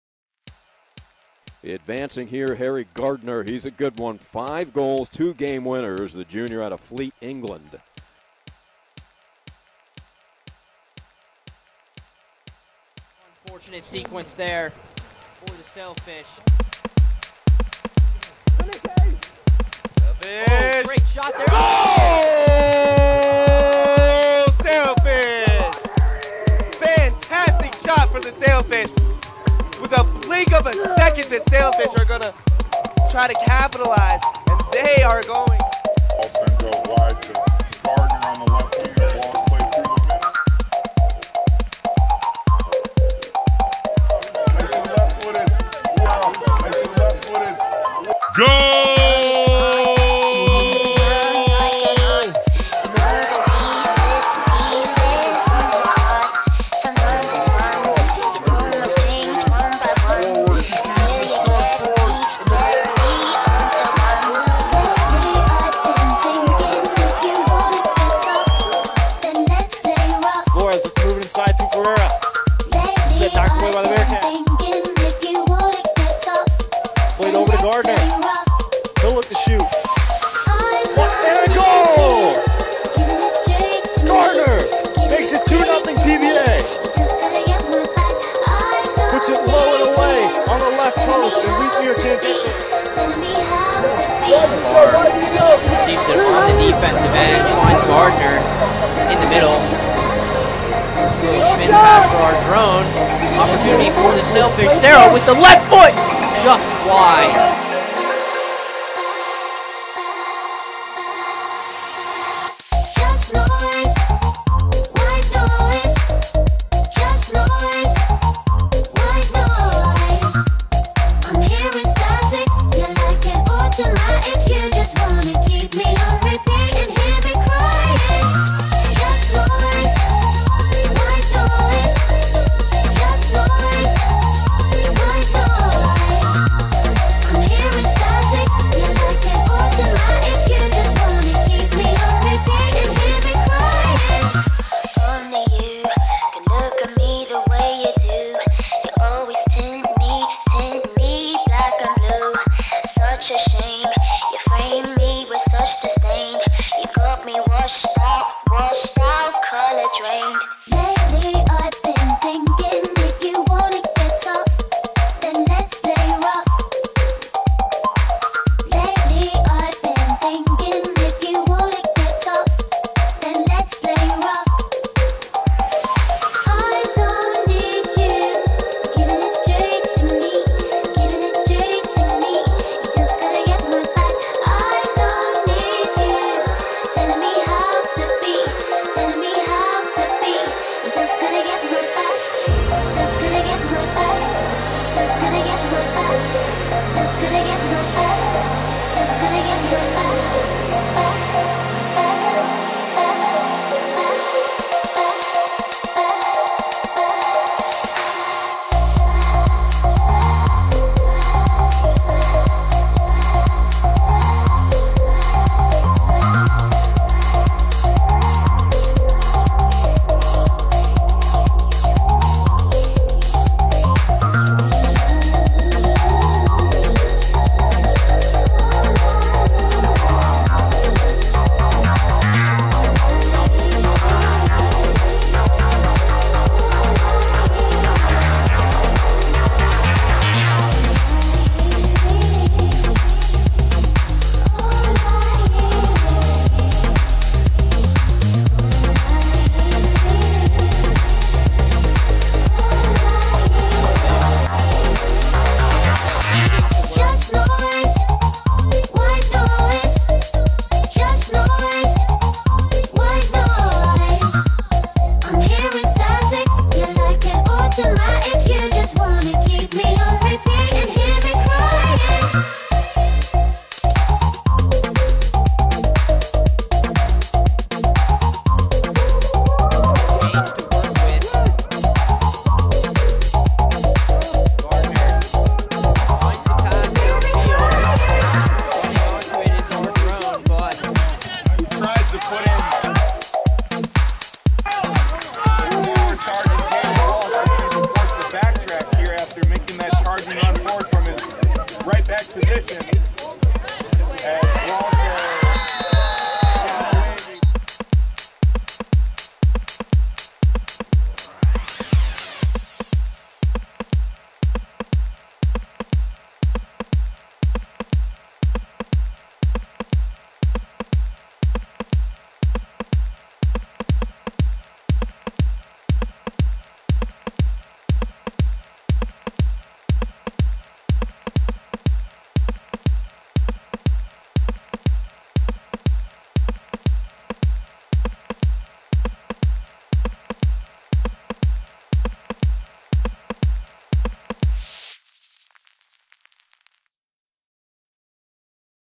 Harry Gardner PBA 2022 Season Soccer Highlights
2022 Sunshine State Conference highlights for Palm Beach Atlantic University

Location: West Palm Beach, Florida

Player Information:
- PBA Soccer Scholar
- Junior
- D.O.B = 01/01/2001
- Height = 6ft
- Right Footed
- Position = Midfielder
- Nationality = British

I do not own the copywrite to this music